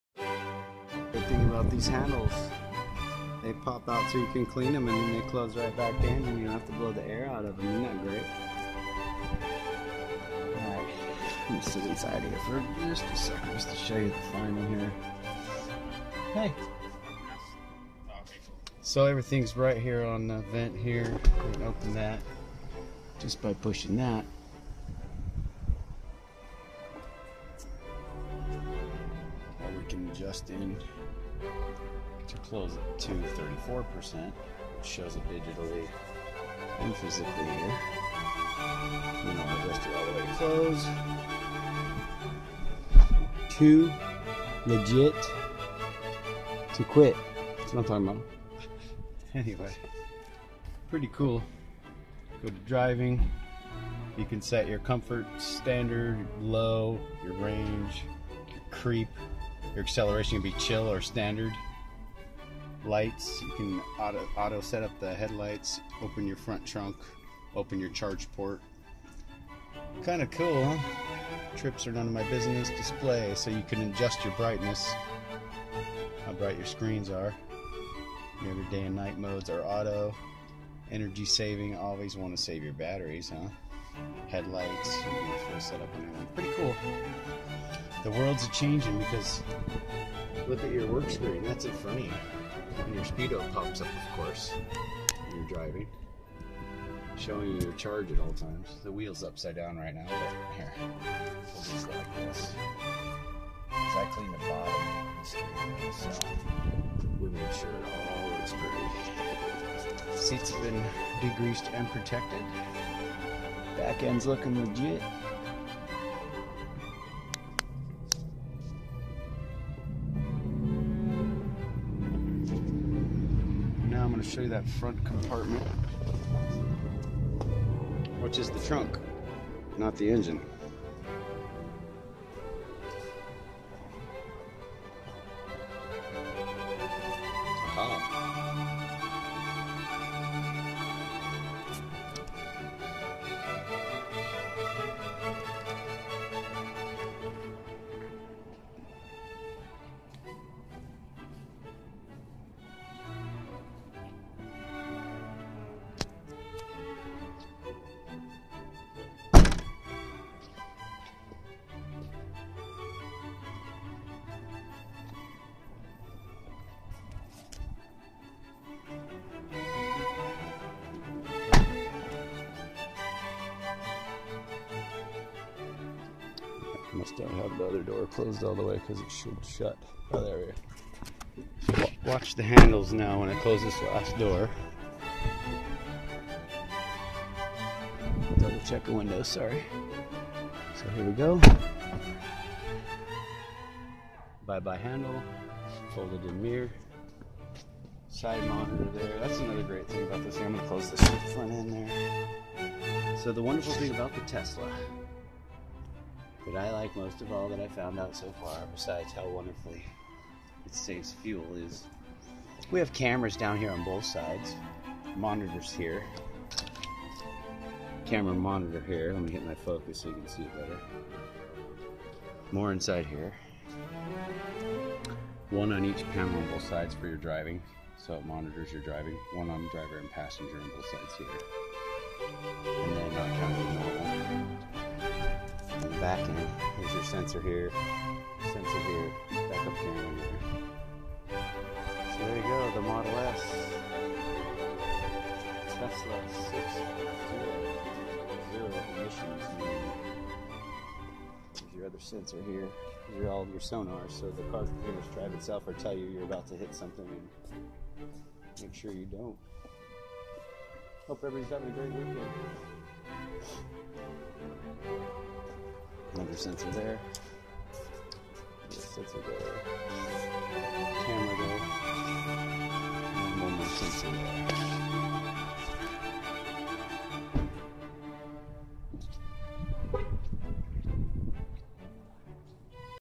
TESLA taking care of one of my favorite clients ride
A walk around of the Tesla six zero  during my Private Clients detail.